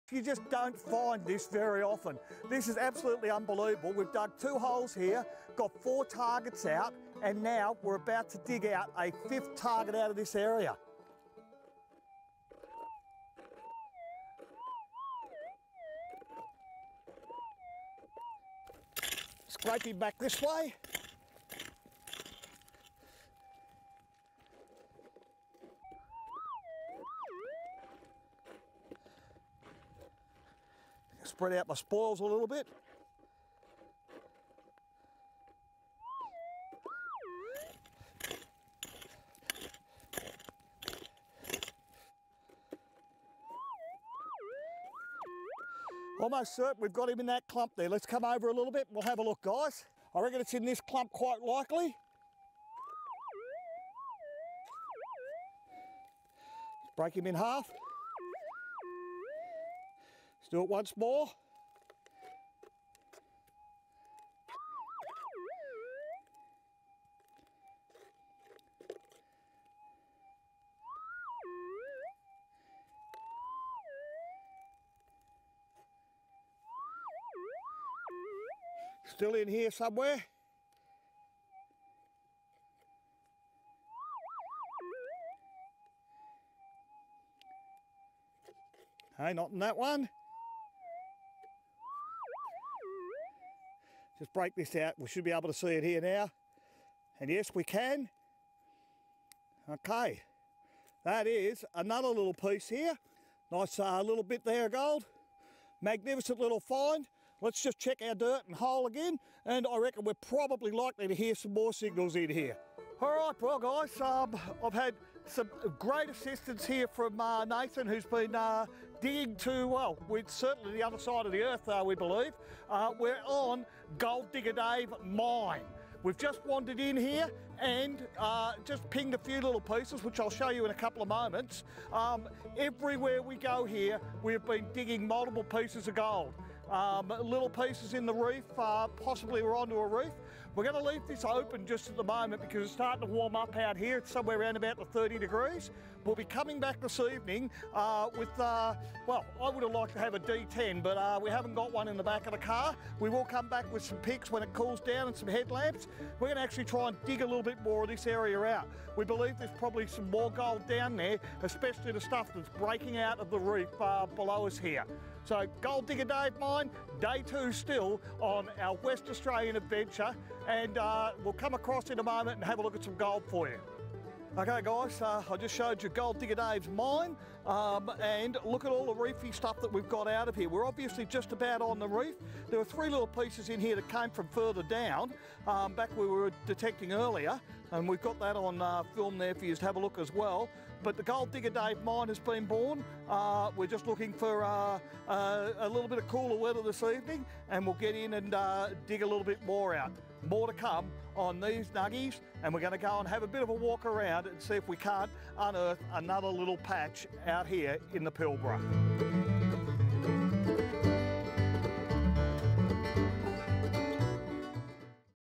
Detecting for Gold in the Pilbara with a Minelab GPX 6000 Metal Detector
Go on a gold prospecting adventure with Gold Digger from The Minelab Show.

Follow Dave on his gold prospecting adventure in the Pilbara, Western Australia.

See how the GPX 6000 metal detector performs in The Pilbara.

Great metal detecting tips for beginners and pros alike.